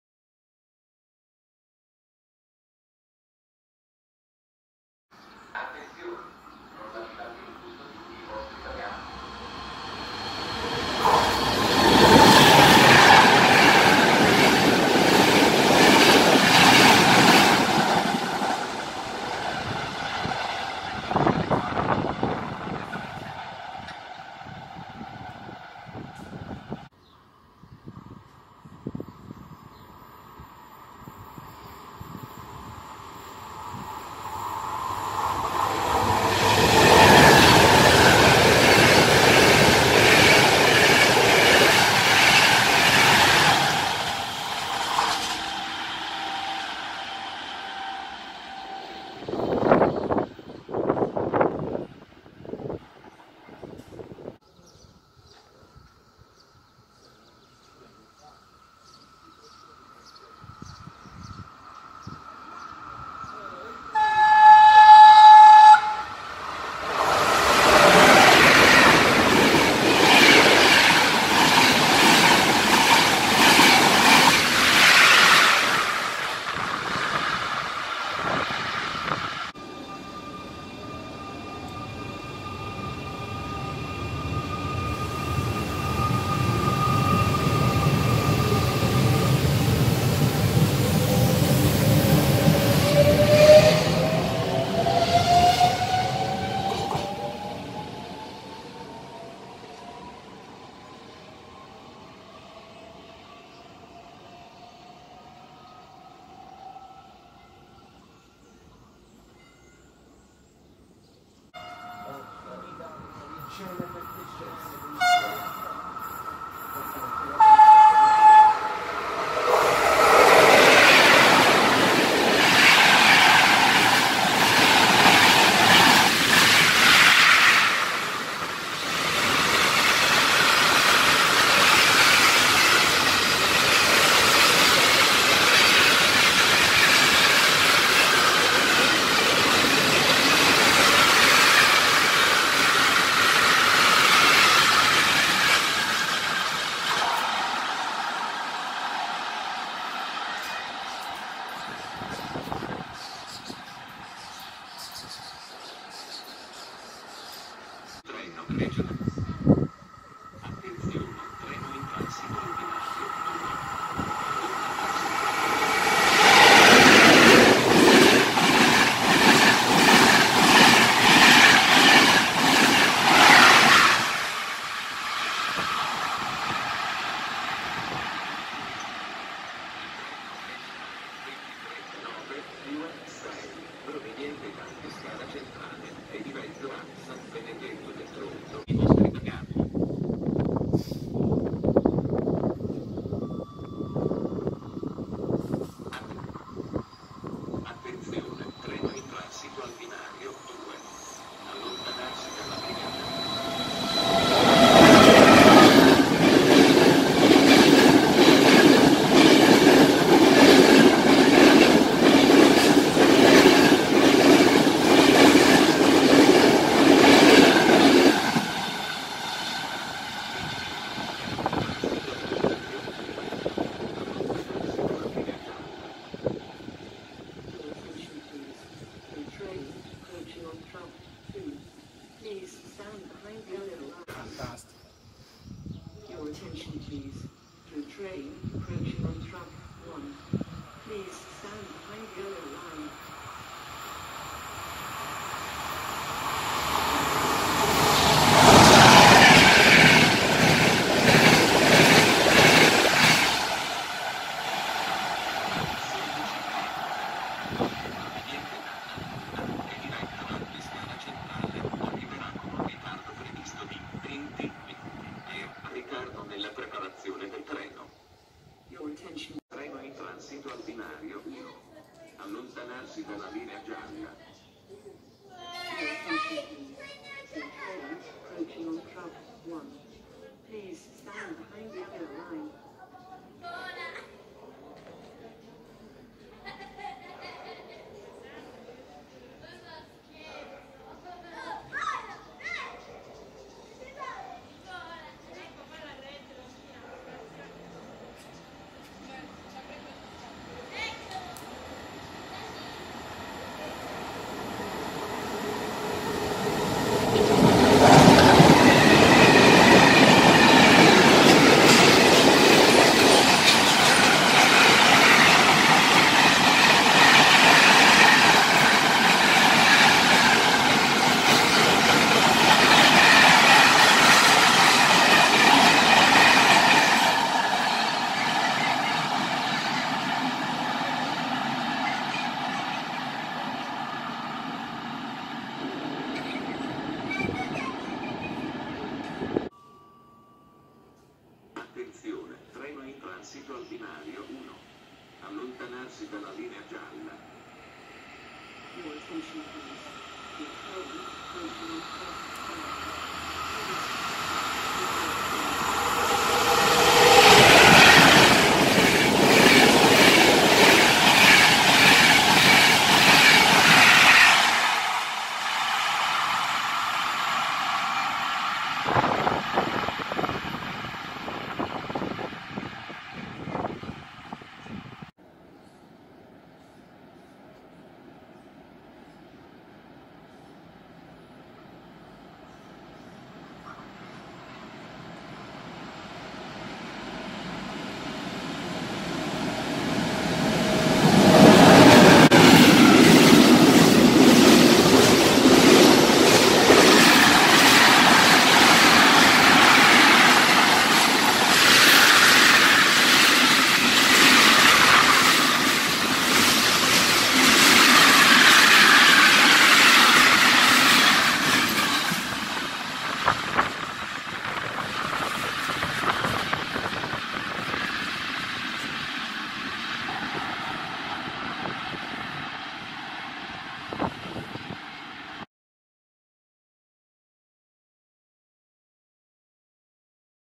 Transiti sulla linea Adriatica tra le stazioni di Alba Adriatica e Tortoreto Lido.
In questo video ci troviamo lungo la linea Adriatica nel tratto al confine tra Marche e Abruzzo e precisamente nelle stazioni di Alba Adriatica e Tortoreto Lido con riprese girate nei giorni 2, 4 e 5 luglio 2019.
Nel video si possono vedere i classici treni che percorrono questa linea, sia passeggeri che merci, tra i quali spicca il transito di una E.444 a fine carriera. In aggiunta ai classici treni della linea merita menzione anche il transito di un Espresso Pellegrini diretto a Lourdes.